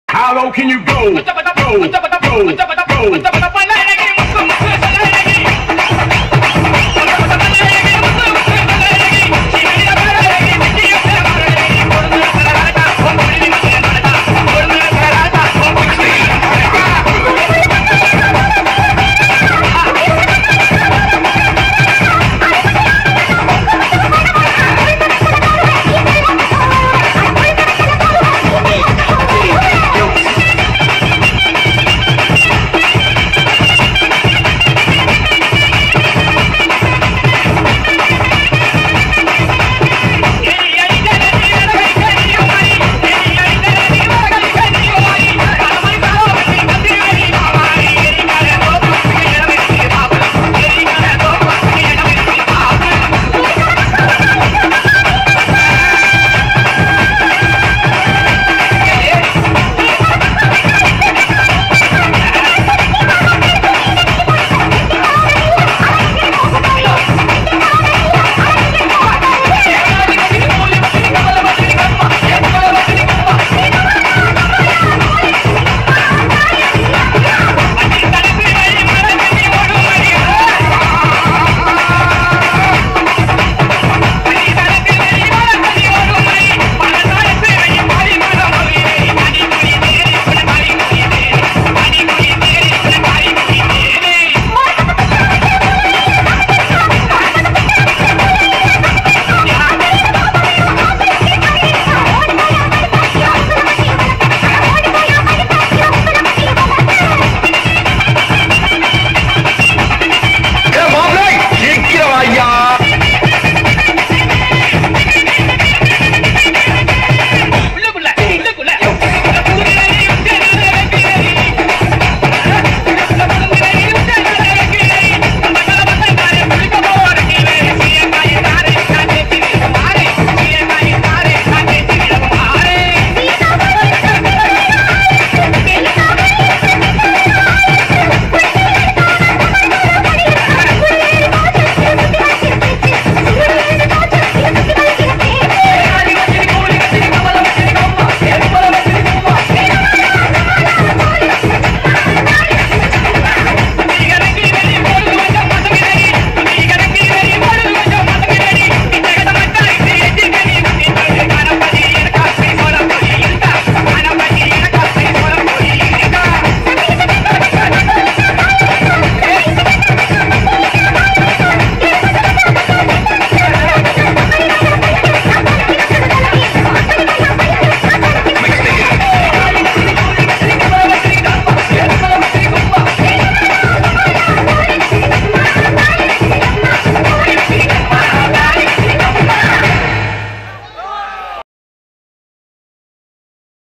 நாகை மாவட்டம் வேதாரண்யம் கரியாப்பட்டினம் ஆடல் பாடல்
கரியாப்பட்டினம் ஸ்ரீ சந்தான மாரியம்மன் கோவில் ஆடல் பாடல் கலை நிகழ்ச்சி